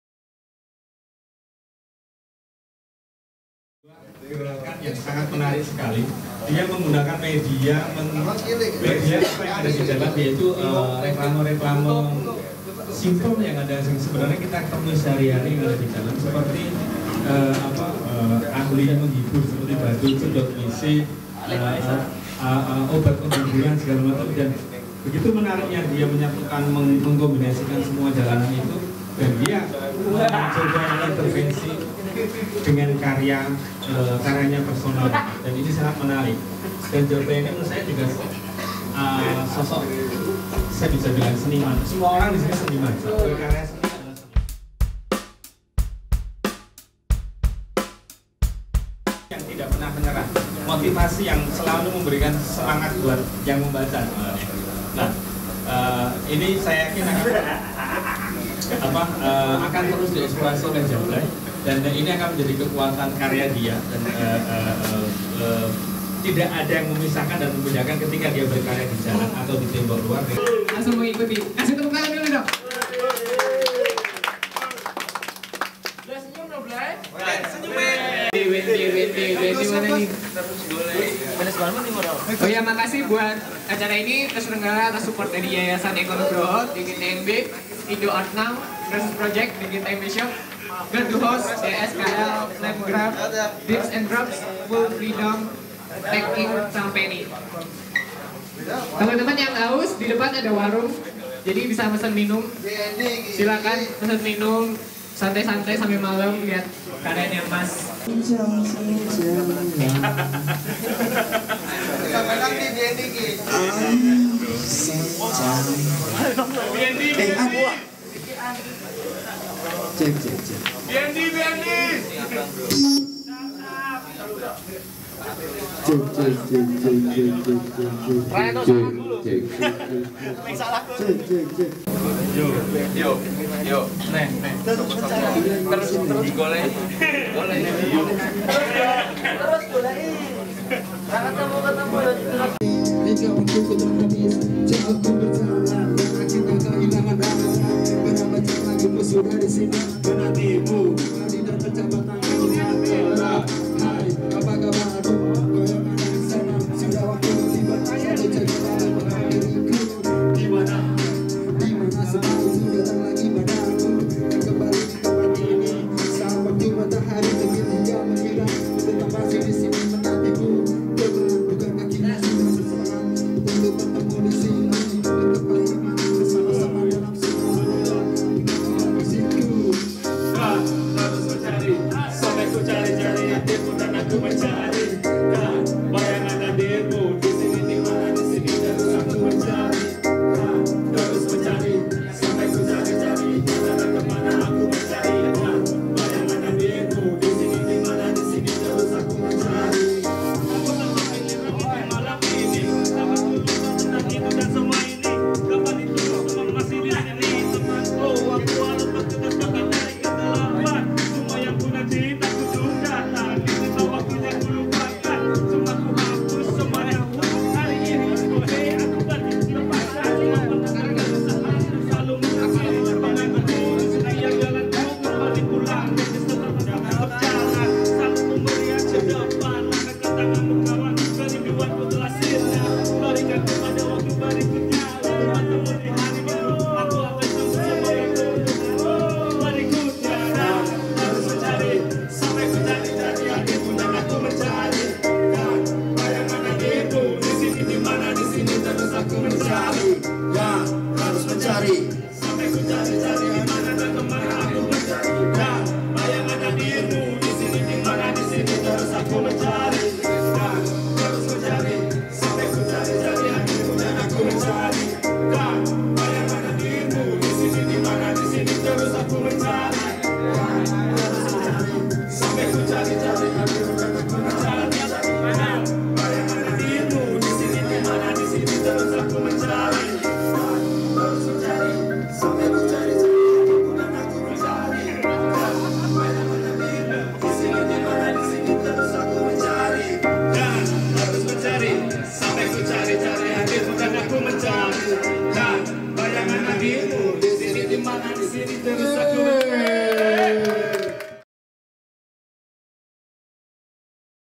Dokumentasi IVAA : DGTMB Versus Project #13 KONTRA KULTUR - Bujangan Urban
DGTMB Versus Project #13
KONTRA KULTUR
Oleh : Bujangan Urban
Kurator : Eko Nugroho

Dimeriahkan Oleh : Begundal Clan & BND

Pembukaan Pameran : Rabu, 23 April 2015
Jam 19:30 WIB – selesai
Di DGTMB Shop, Jalan Kadipaten Kidul 28
Yogyakarta

video Ika Nurcahyani
editing Dwi Rahmanto
Video Koleksi Indonesian Visual Art Archive
2015